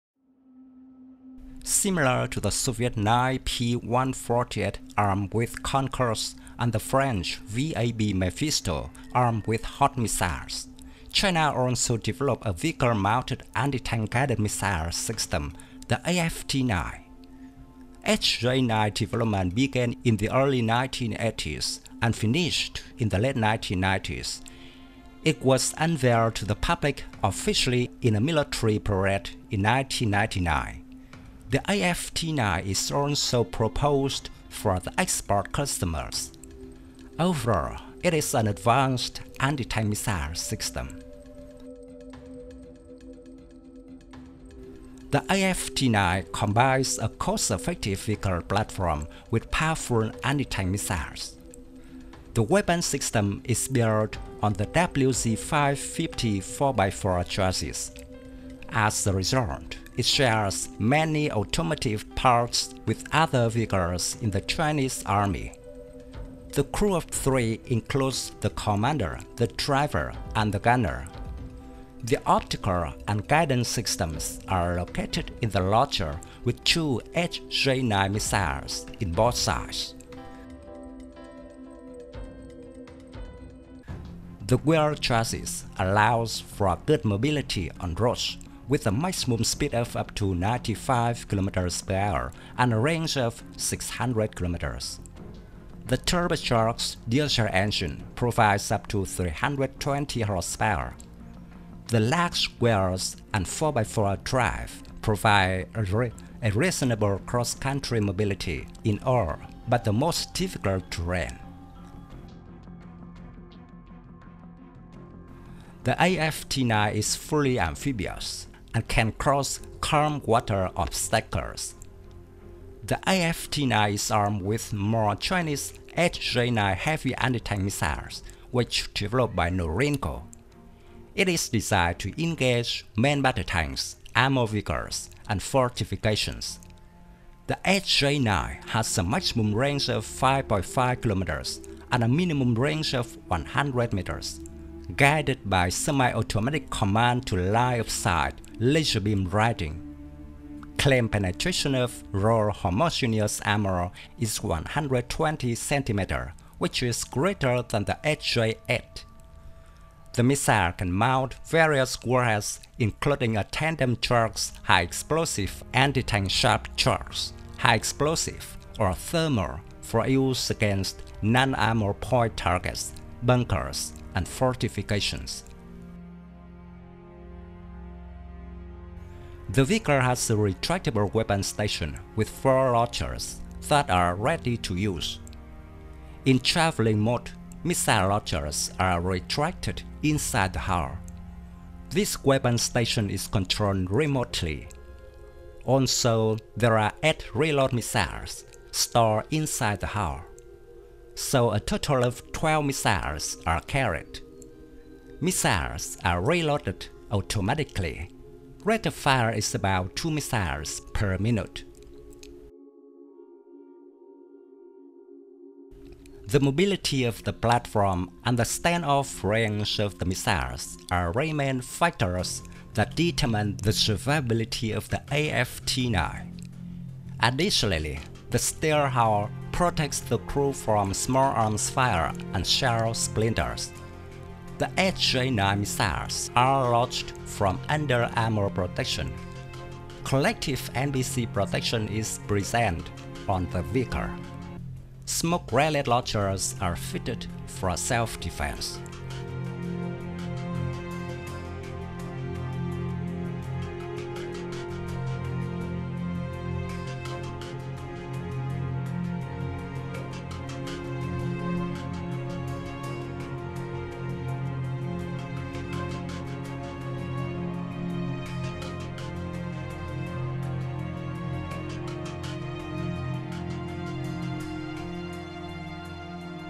AFT-9 - Chinese Modern Tank Destroyer
Similar to the Soviet 9P148 armed with Konkurs and the French VAB Mephisto, armed with HOT missiles, China also developed a vehicle-mounted anti-tank guided missile system, the AFT-9. HJ-9 development began in the early 1980s, and finished in the late 1990s. It was unveiled to the public officially in a military parade in 1999. The AFT-9 is also proposed for the export customers. Overall it is an advanced anti-tank missile system.

Thank you very much.
Best regards
Dung Tran.